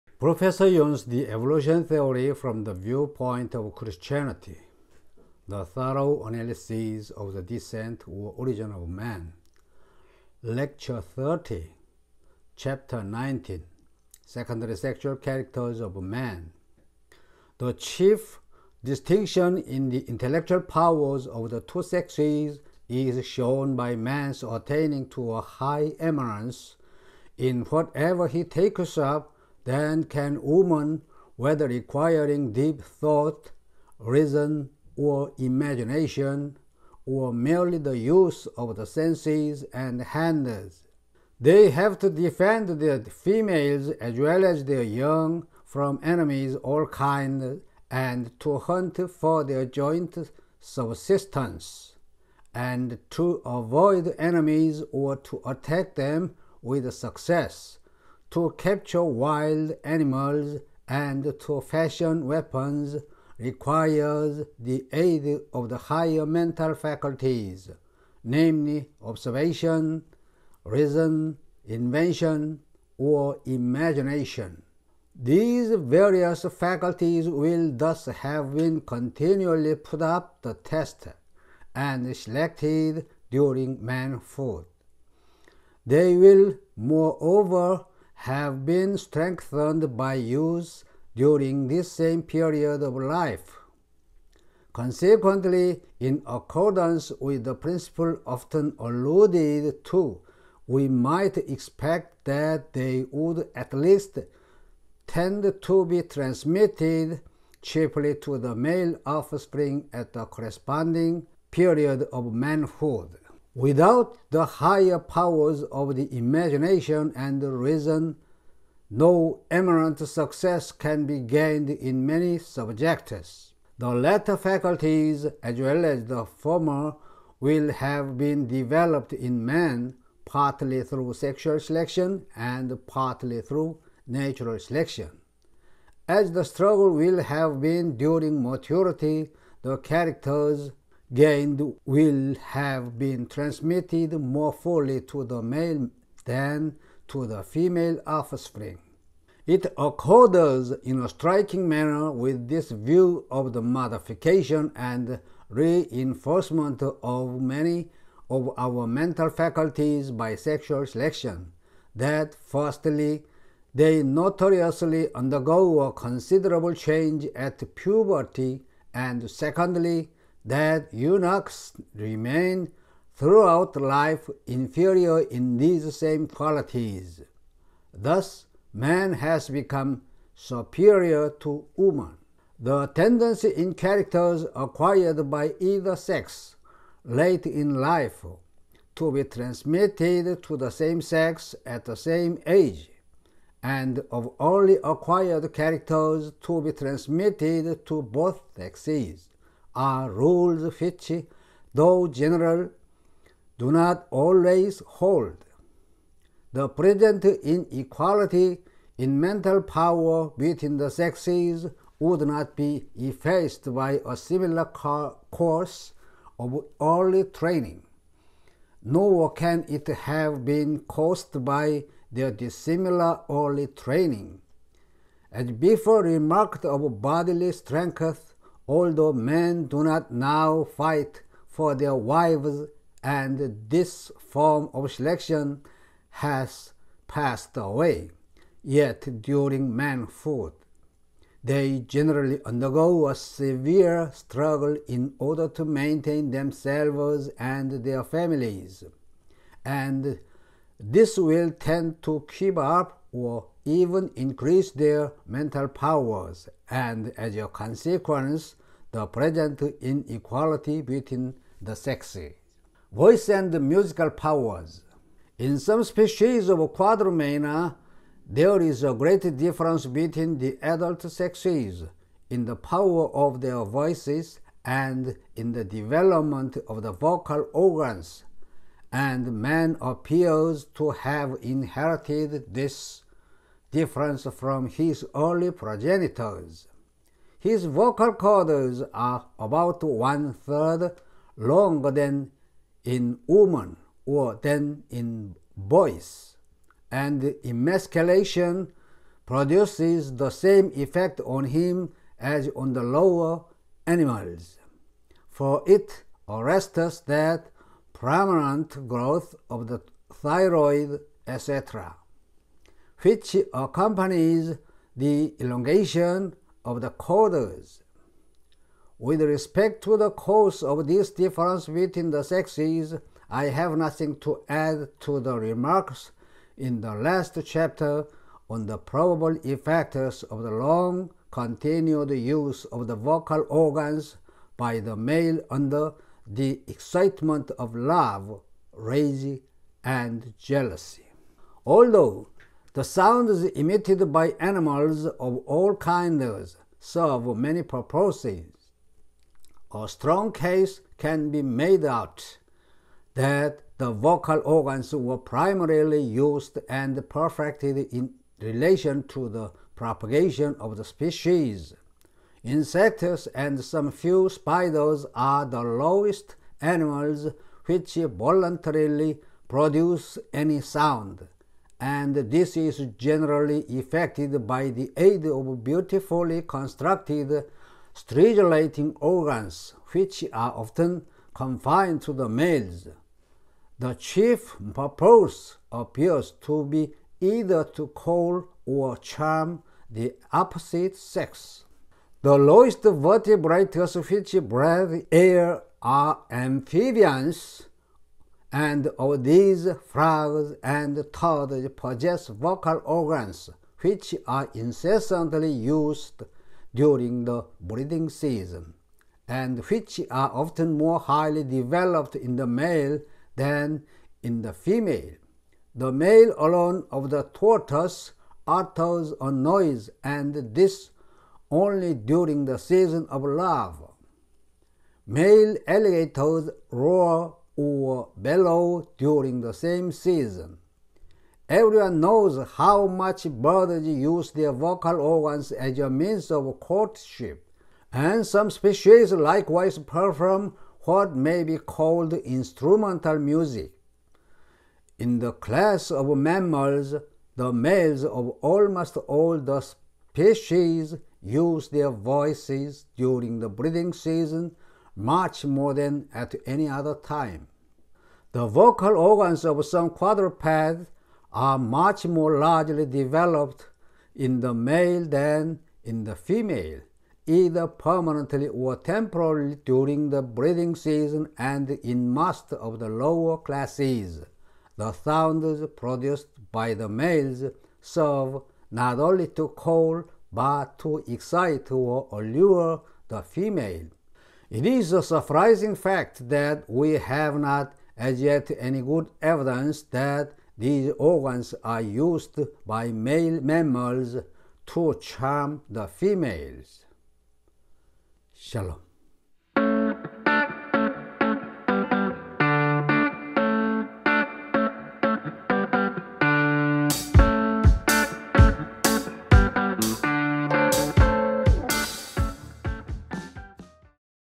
Lecture 30. CHAP. 19, Secondary sexual characters of man (continued)
It accords in a striking manner with this view of the modification and re-inforcement of many of our mental faculties by sexual selection, that, firstly, they notoriously undergo a considerable change at puberty, and, secondly, that eunuchs remain throughout life inferior in these same qualities.

Thumbnail : Courtesy Mark S. Howard